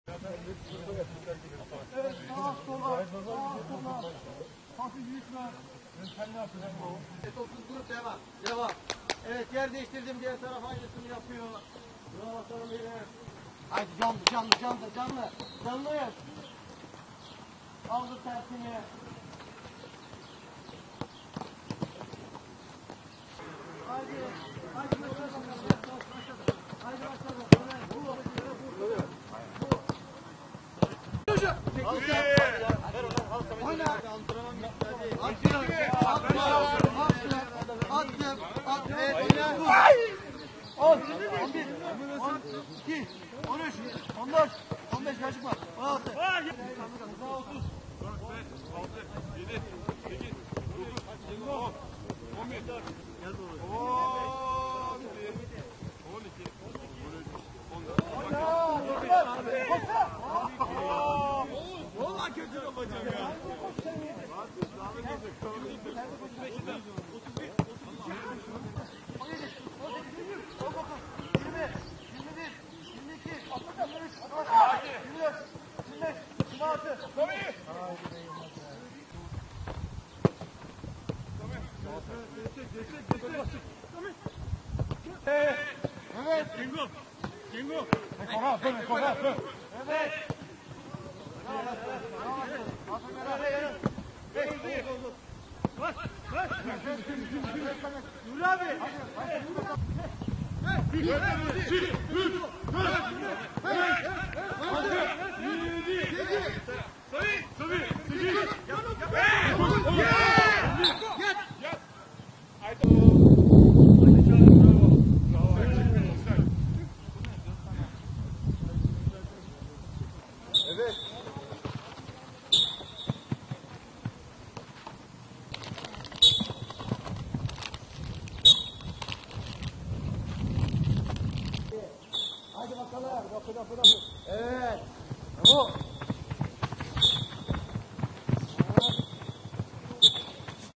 YILPORT SAMSUNSPOR'DA HACETTEPE HAZIRLIKLARI TAMAMLANDI
Yılport Samsunspor Takımı, yarın deplasmanda karşılaşacağı Hacettepe maçı hazırlıklarını bugün (12 Nisan cuma) Ankara’da yaptığı antrenmanla tamamlayarak maç saatini beklemeye başladı.